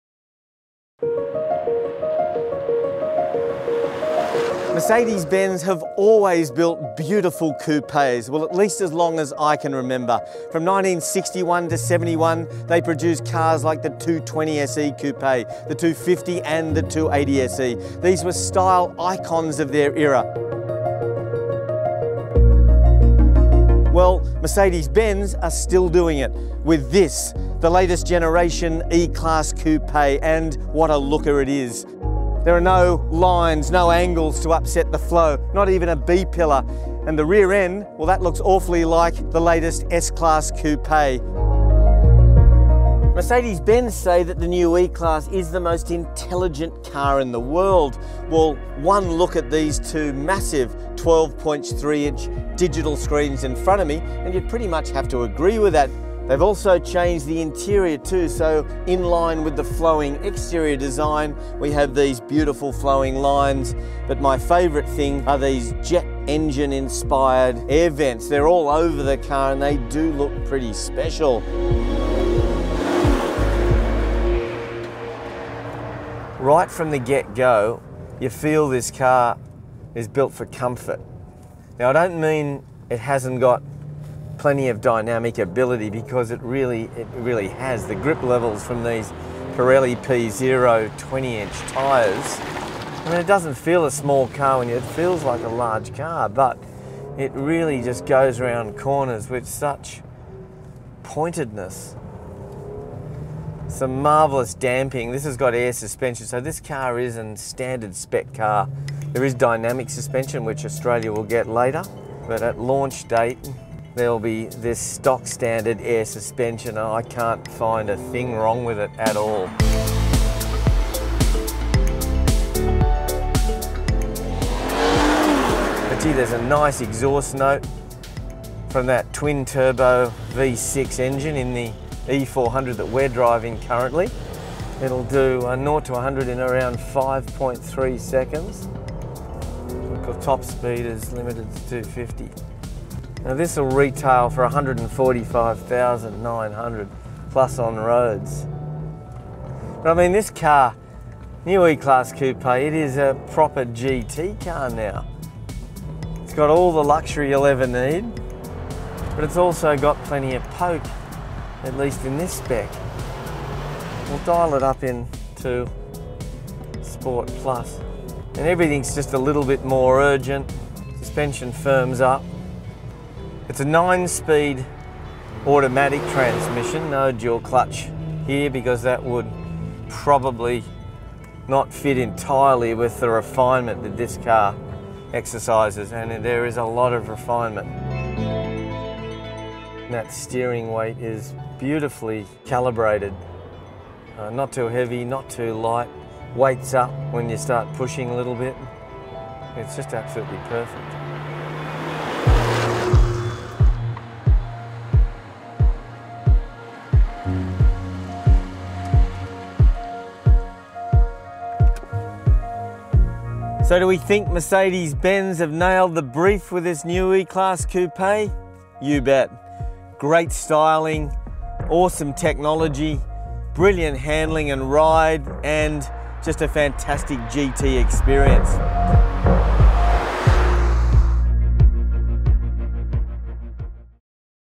2017 Mercedes-Benz E-Class Coupe First Look review | CarAdvice
Mercedes-Benz has been building jaw-dropping coupes for decades, or at least, for as long as I can remember.

From 1961-71 was a particularly creative period for the German luxury car manufacturer, which saw the likes of the 220SE, 250SE and 280SE coupes released. These were beautifully proportioned two-door luxury rides that quickly found favour with the rich and famous and became style icons of that era.